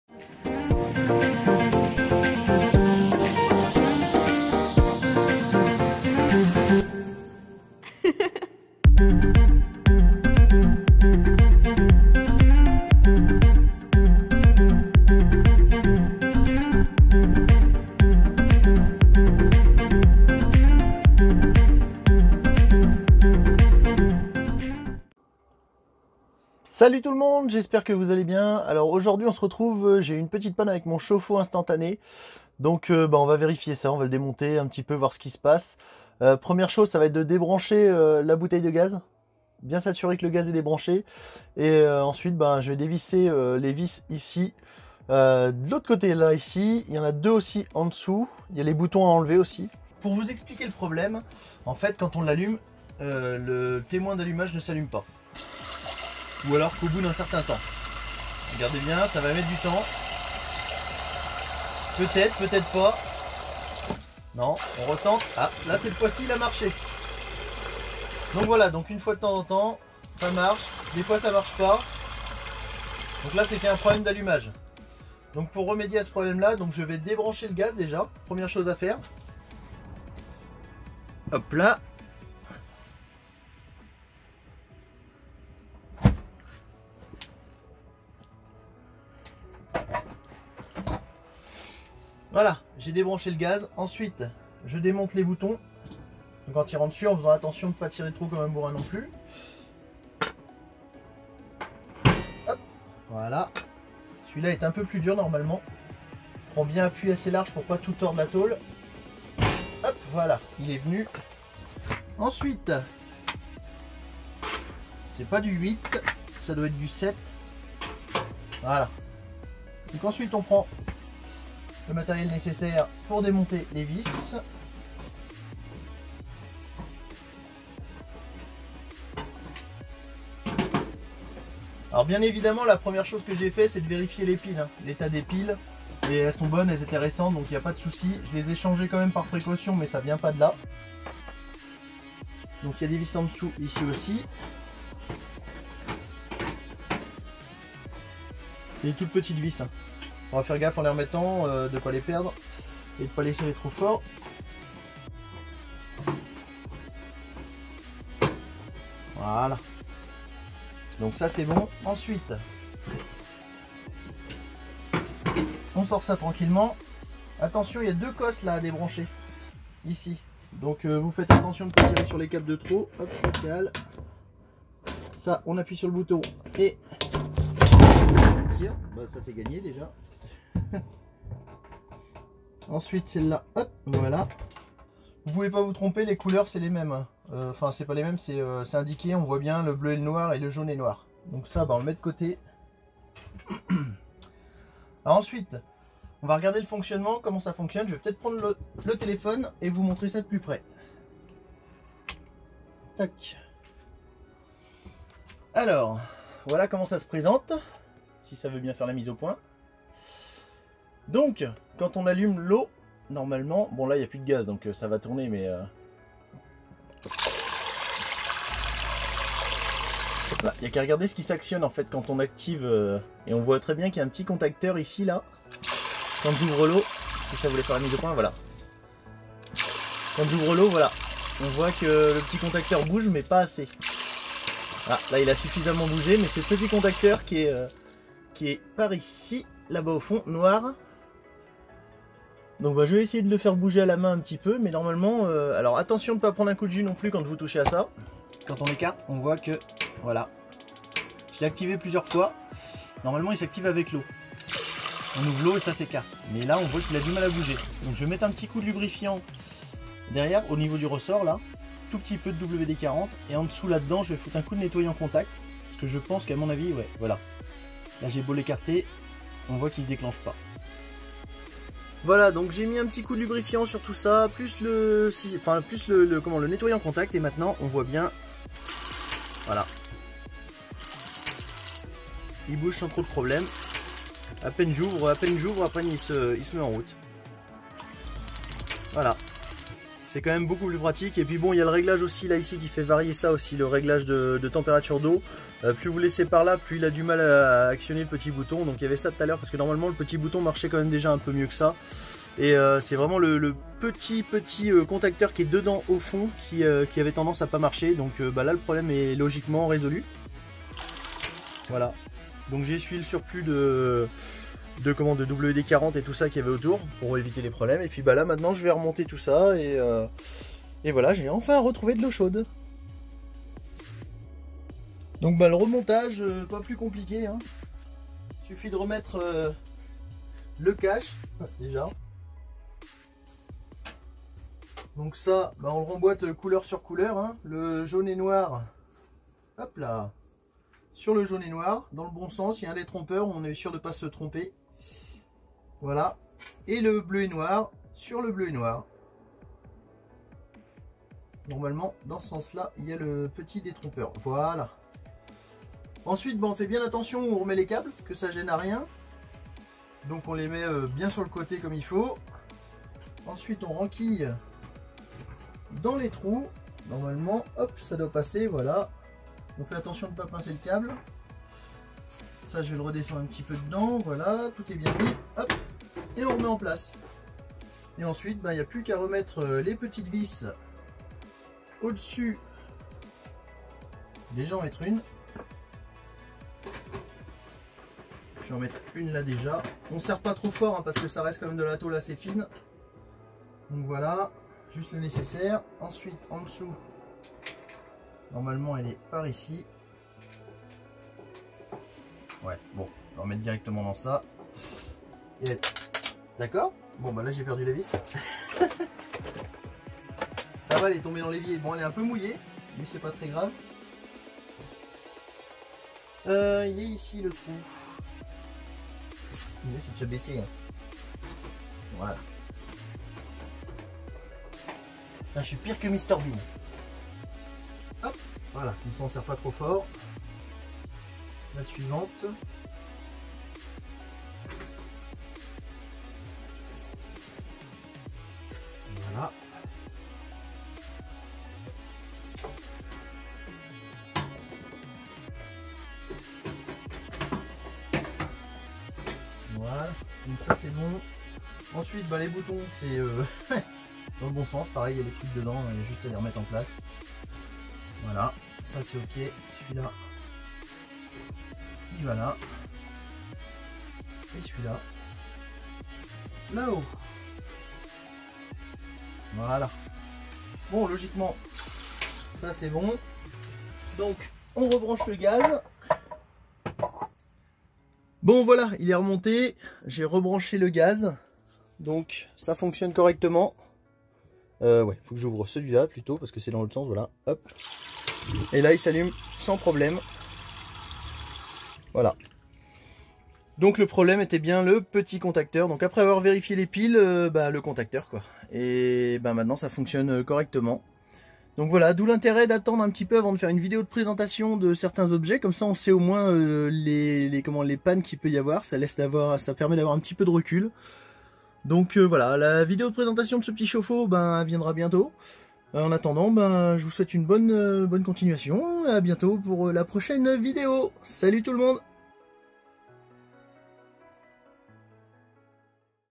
Panne d'allumage Chauffe Eau Gaz - RIDGEYARD / FASTAR
Petit souci d'allumage du chauffe eau Ridgeyart (équivalent FASTAR)
Résolue très facilement 😊

Lien du chauffe eau :